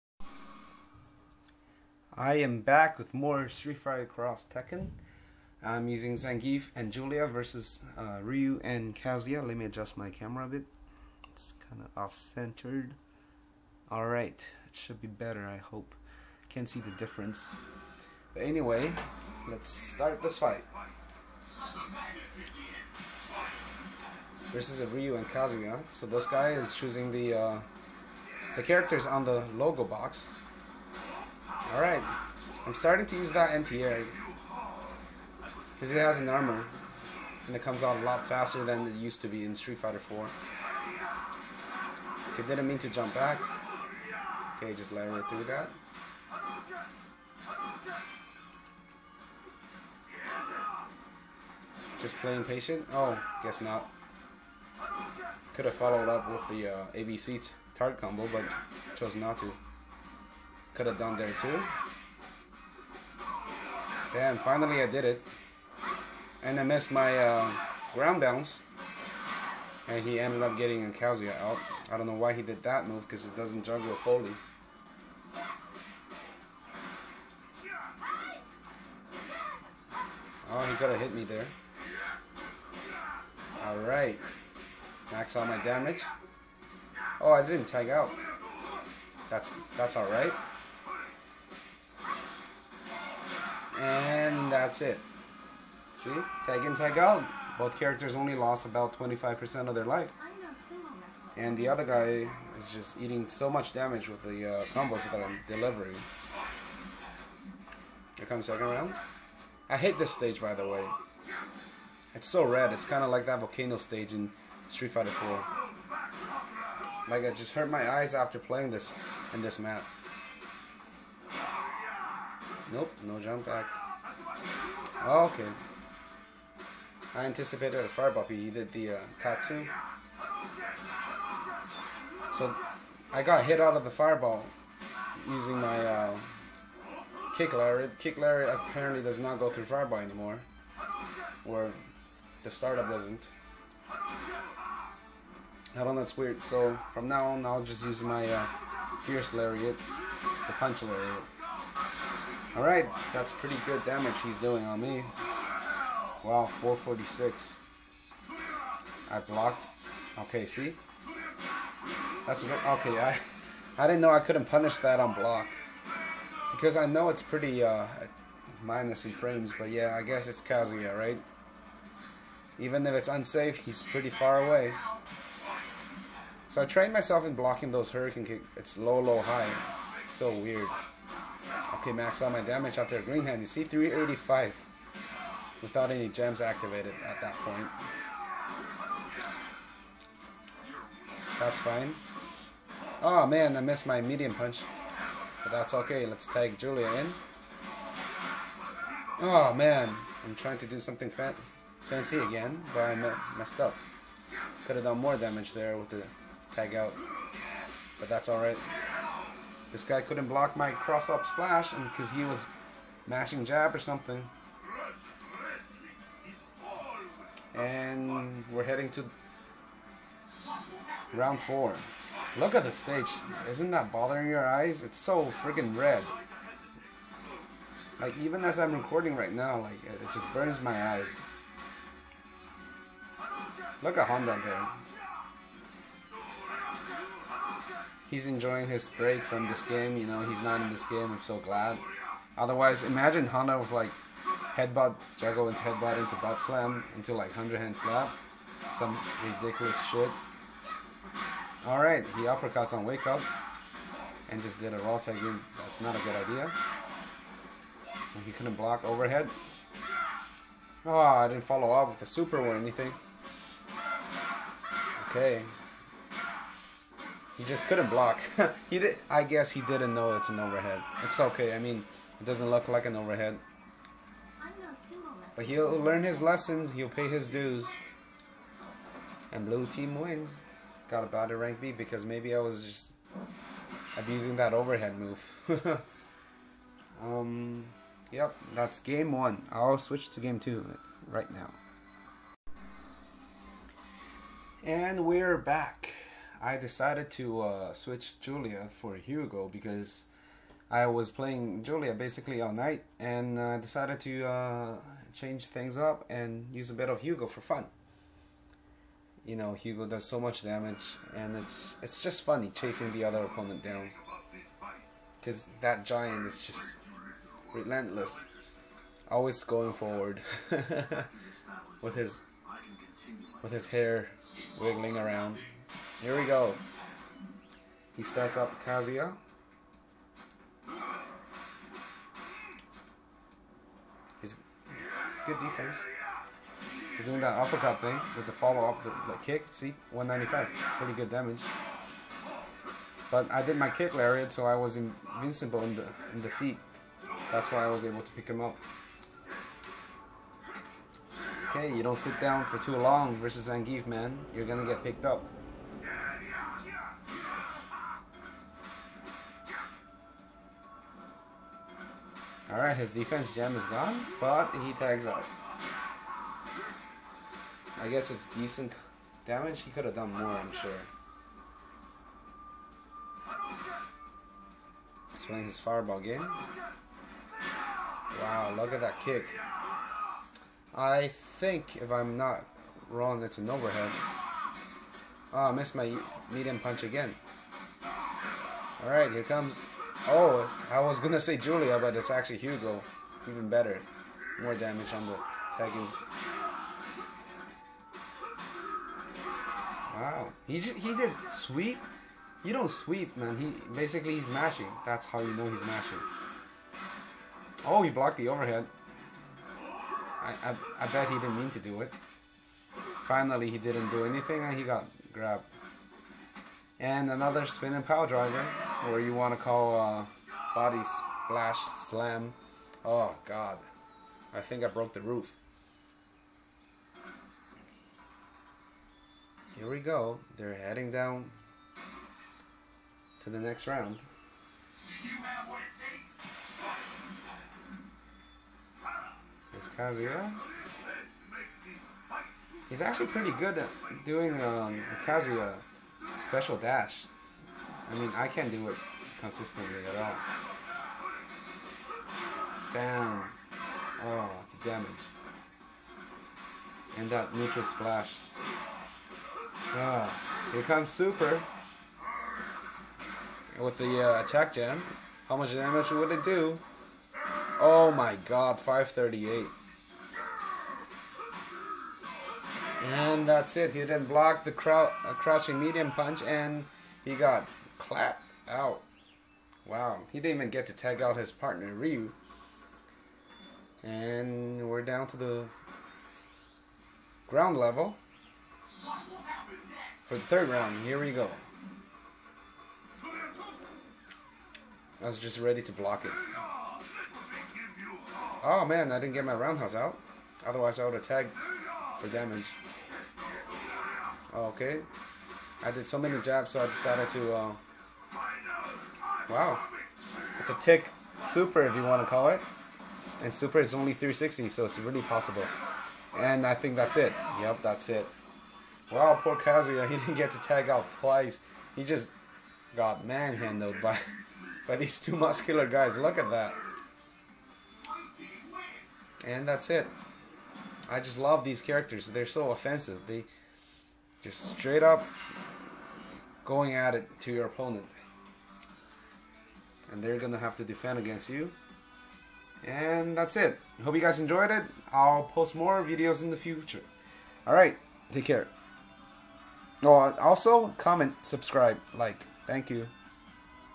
Julia, Hugo, Zangief (me) vs. Kazuya & Ryu
Another few games of me using Julia, Hugo, and Zangief teams versus Kazuya & Ryu